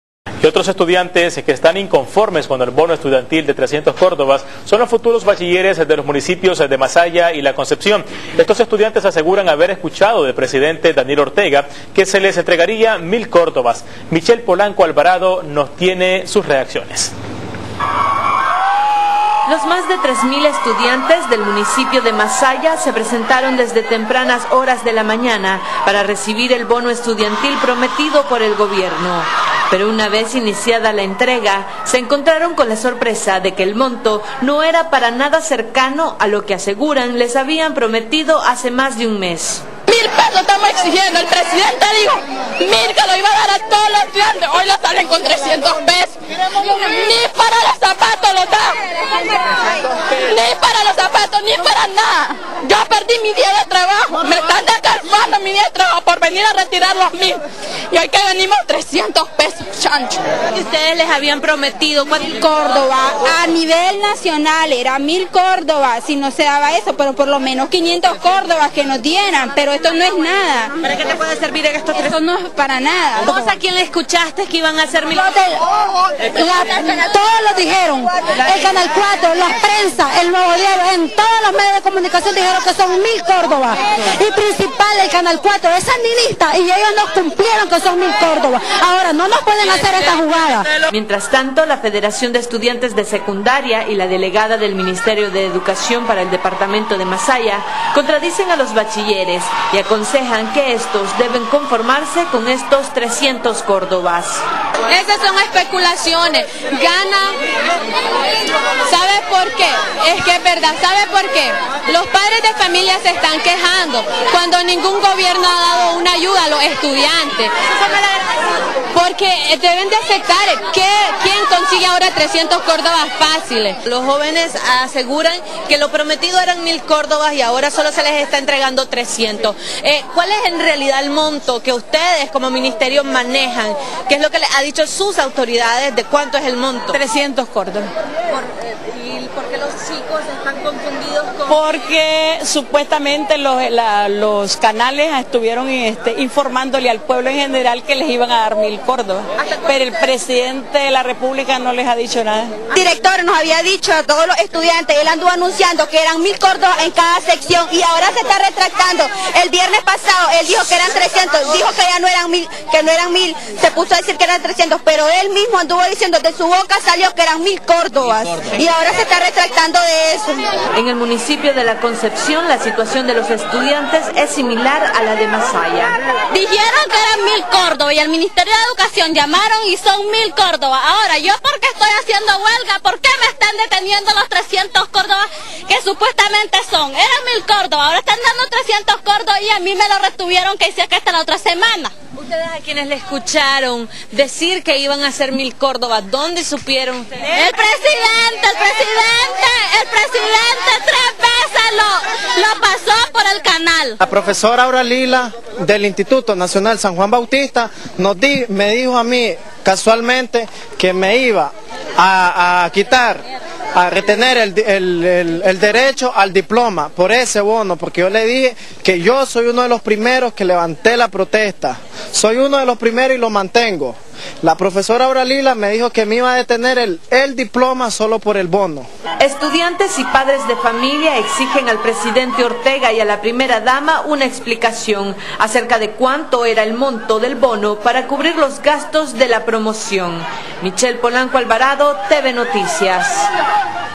Bachilleres inconformes con bono que les dio el gobierno - Parte 2
Aseguran que les prometieron, en campaña electoral, C$1,000 córdobas.